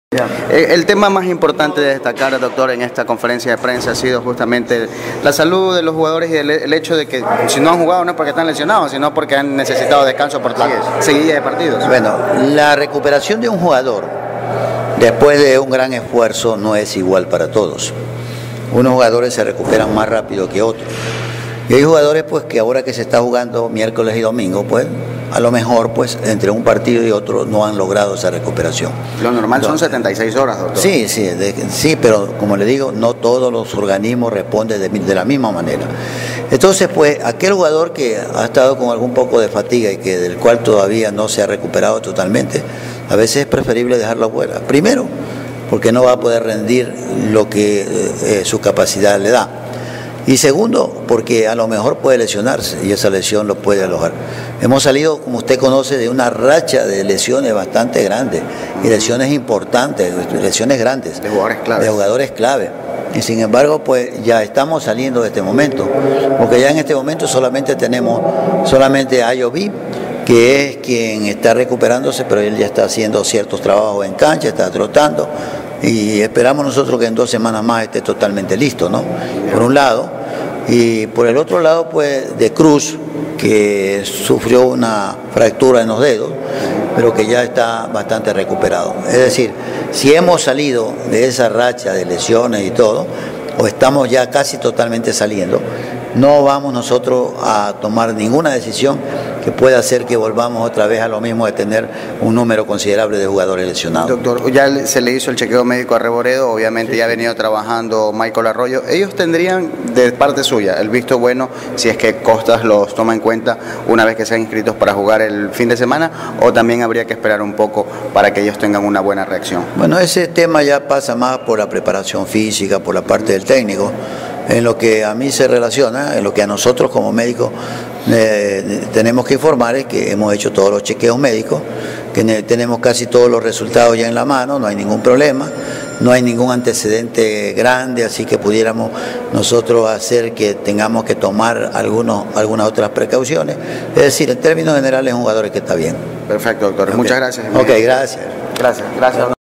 Dr. Mendoza: "Barcelona está saliendo de sus lesionados"
El Dr. Bosco Mendoza anunció q Barcelona ya no tiene casi jugadores lesionados. Audio radio Huancavilca